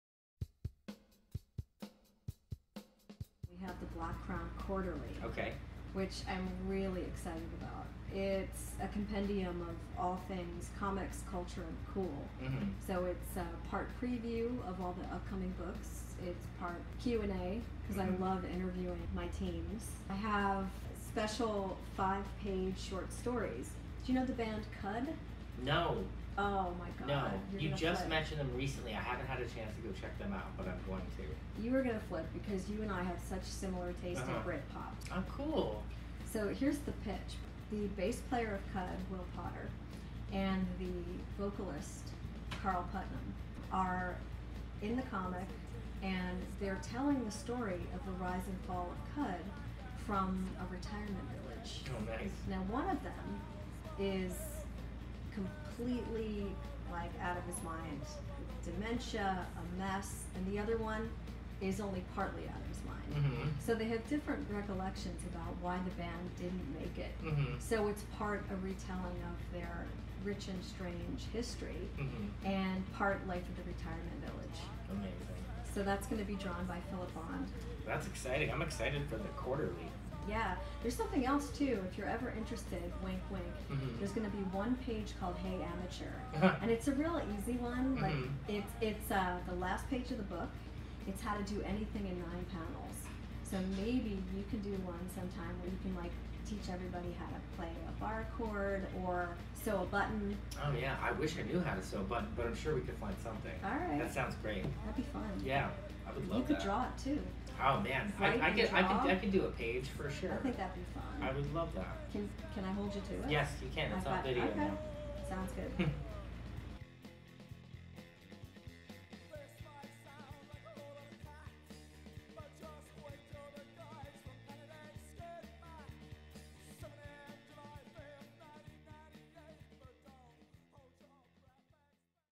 BLACK CROWN QUARTERLY
Black Crown Editor Shelly Bond talks with Gerard Way about the Black Crown Quarterly, available 10/25/17.

Everything you always wanted once a season is packed within these 48 pages including a two-sided pullout poster and a wraparound cover!

Delight in regular continuing features like legit publications with literary pedigrees! It all begins with the 10-page regular lead feature, Tales From the Black Crown Pub starring Stacey the barmaid by Award-winner Rob Davis (The Motherless Oven). Recurring short features include Canonball Comics: an exquisite corpse that will not stay dead kicked off by Jamie Coe (Artschooled) and Swell Maps by respectably divine music journalist/novelist Cathi Unsworth.

Plus: Space CUDets rejoice: Live from a posh retirement village for wannabee 4-hit wonders we have co-writers/bandmates Will Potter and Carl Puttnam and occasional artist Philip Bond.

Plus Plus: Special previews, creator interviews and behind the panel border secrets from Kid Lobotomy, Assassinistas, Punks Not Dead, and much, much more!

Featuring a pull-out poster! One side is a street map, the other is Frank Quitely’s Kid Lobotomy #1 cover!

Plus, a wraparound cover by Rob Davis!